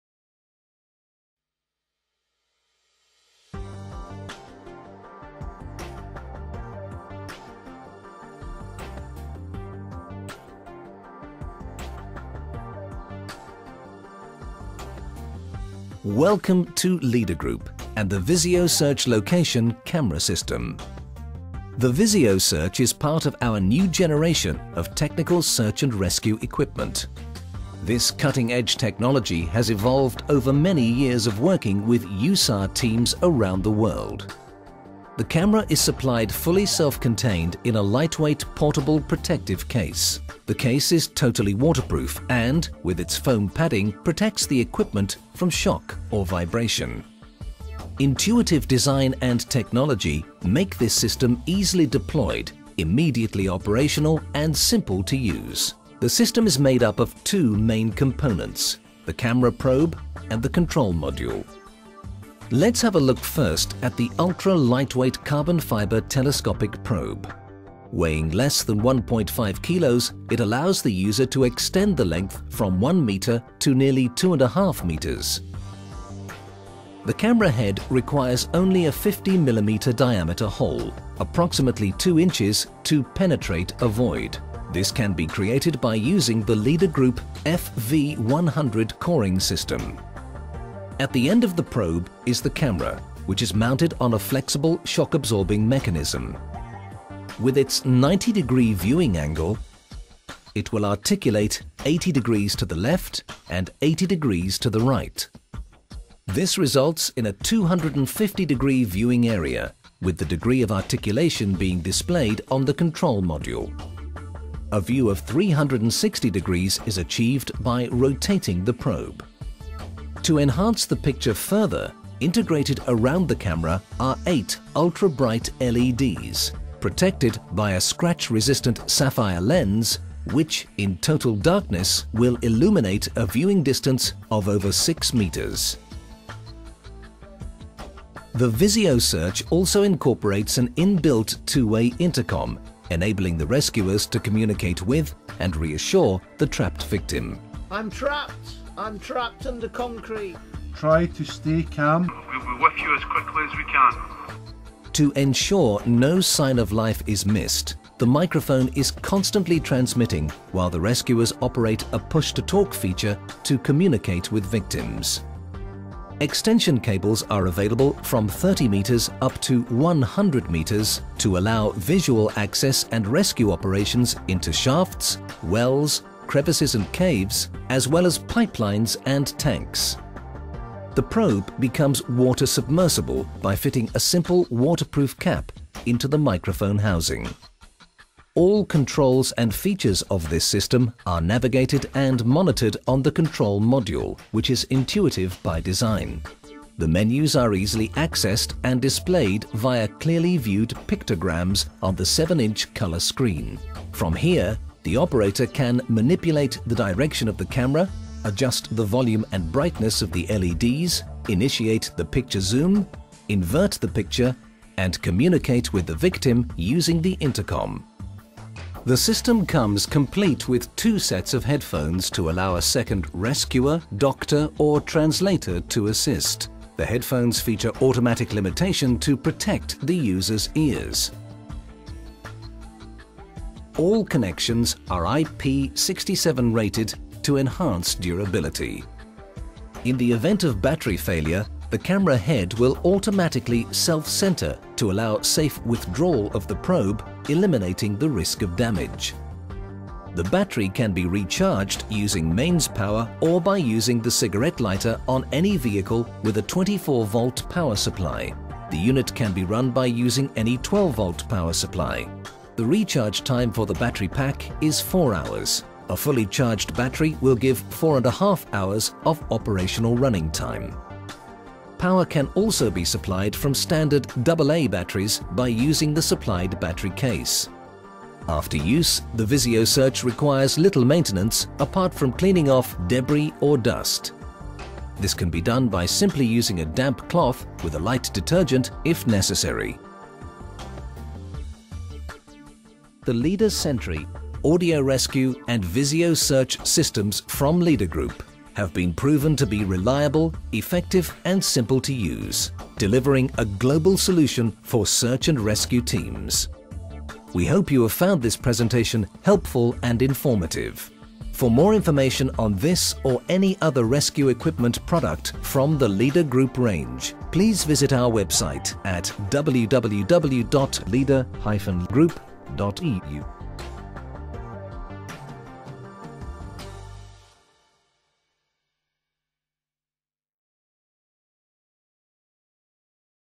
LEADER Visio Search life locator camera for search and communication
VISIO SEARCH - Search and communication camera
Life locator for trapped victims... Visually locate buried victims and talk to them.

With a colour camera on an ultra lightweight boom in connection with a bright and extra wide screen, VisioSearch allows rescue teams to discover and to visualize over 360° exact victims position trapped under rubble.

In addition in Search & Rescue responses in various conditions, VisioSearch also successfully provides clear and corrects images for:

Maintenance operations in industry and construction
Customs inspections
Police investigations

VISIO SEARCH : Communication et visualistaion de victimes ensevelies
Caméra haute définition de recherche de victimes

Le VISIO SEARCH a été développé pour visualiser avec précision, évaluer le niveau d'assistance et de secours nécessaires et dialoguer avec les victimes.